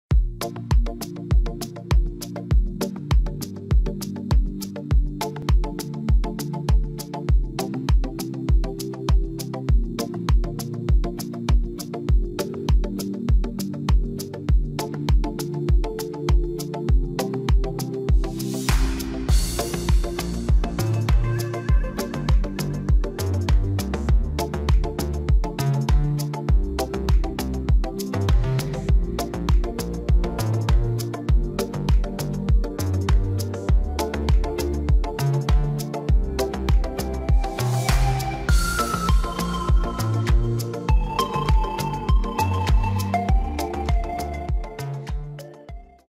Quick Page Composition with Photo Masks and Texts in Album Xpress By DgFlick
Check out How easily you can do your Page Composition with Photo masks and texts to make your album page more beautiful in Album Xpress.

For Sales: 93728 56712, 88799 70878

For Support: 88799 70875, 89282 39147

Address: 603 Techno IT Park, New Link Rd, Eksar, Borivali West, Mumbai, Maharashtra 400091.